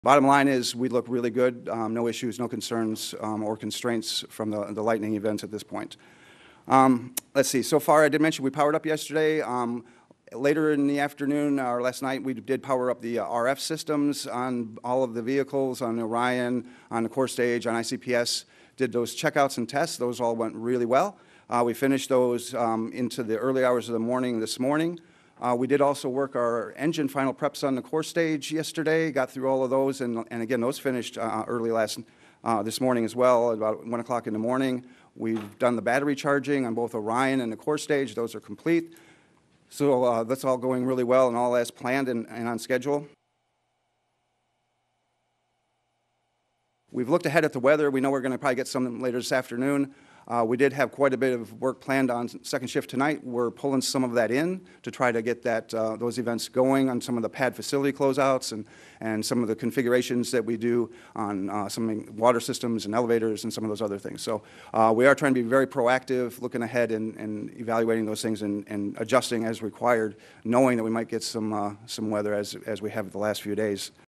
'We look really good' -NASA sees no issues before launch
Scientists at NASA say the SLS-Orion spacecraft blastoff from the Kennedy Space Center looks favorable, barring weather disturbances or unforeseen technical difficulties.